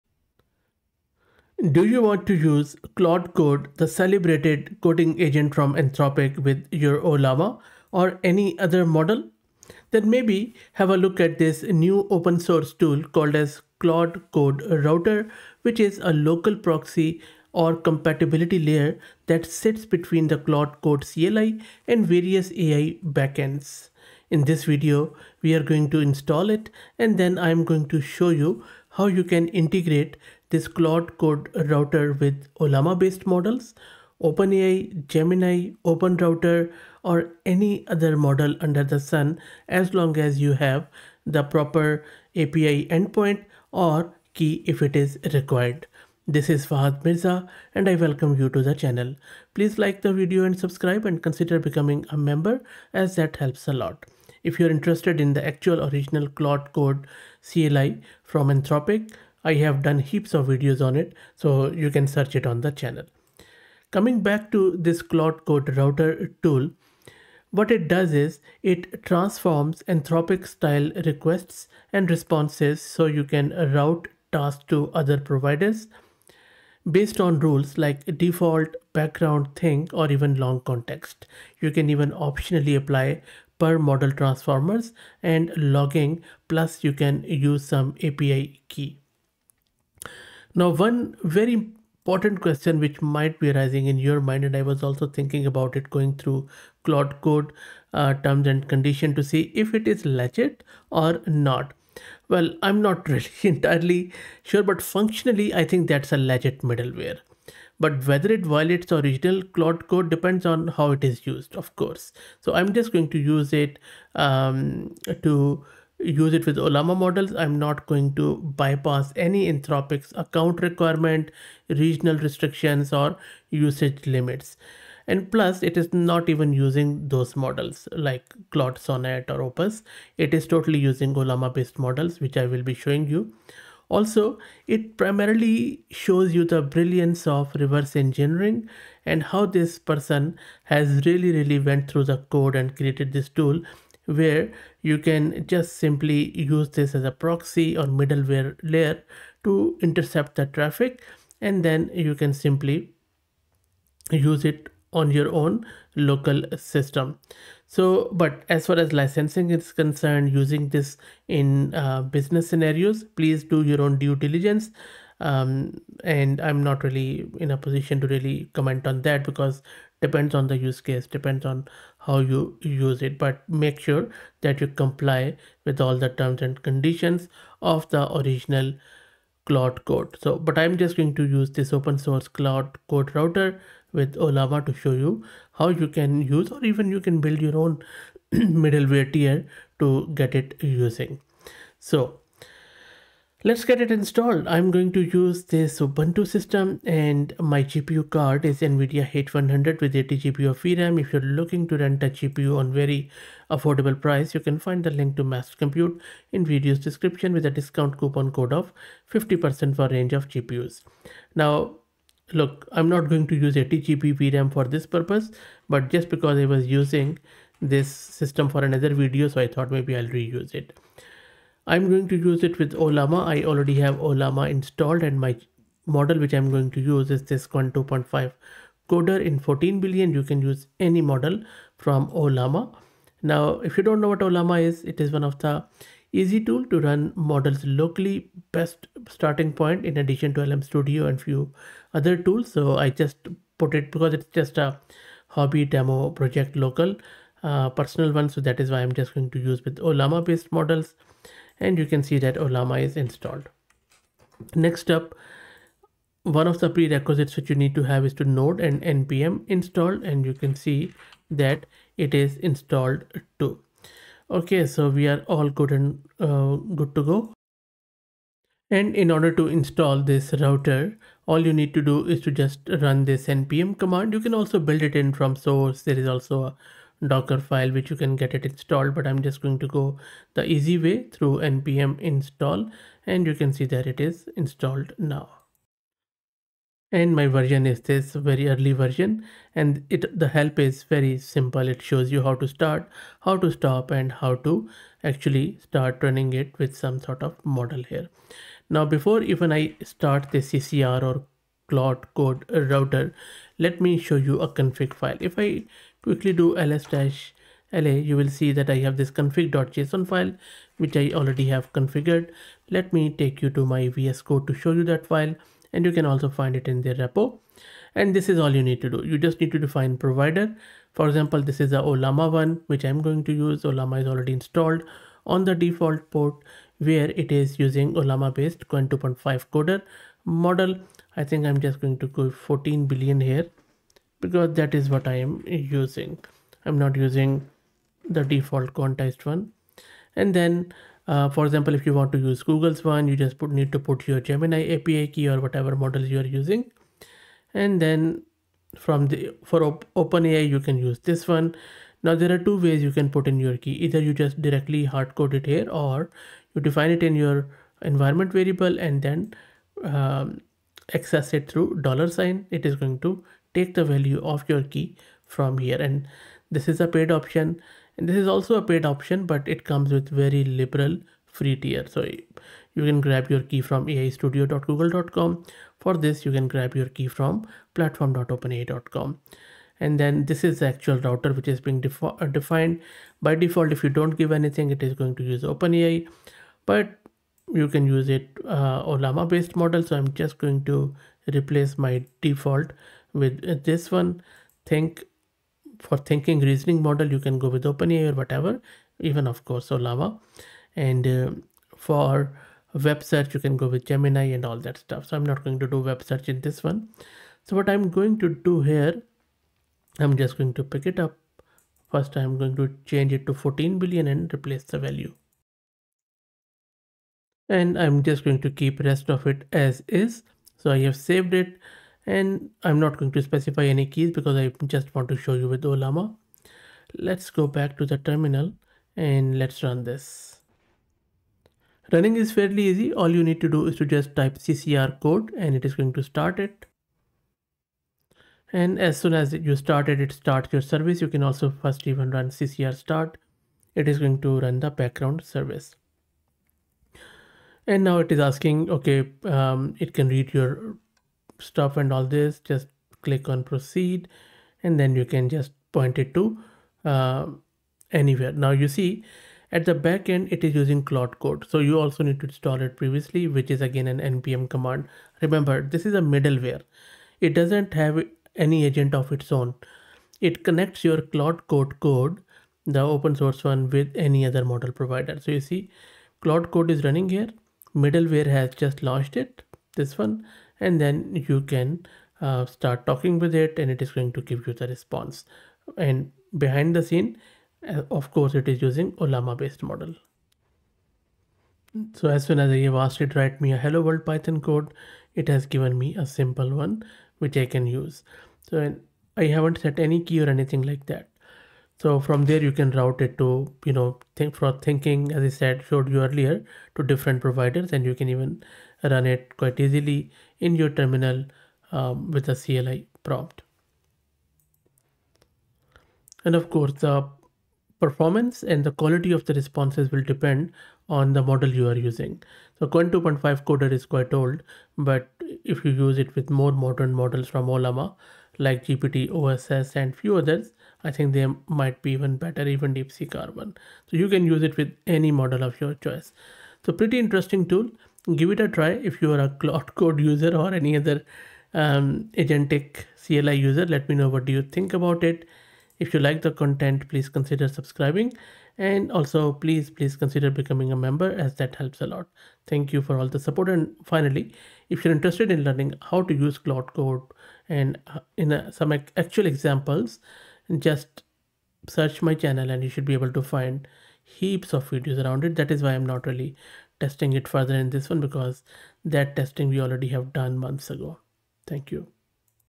Use Use Claude-Code with Ollama with Router Locally
This video installs Claude-Code-Router with Ollama, which is used to route Claude Code requests to different models and customize any request.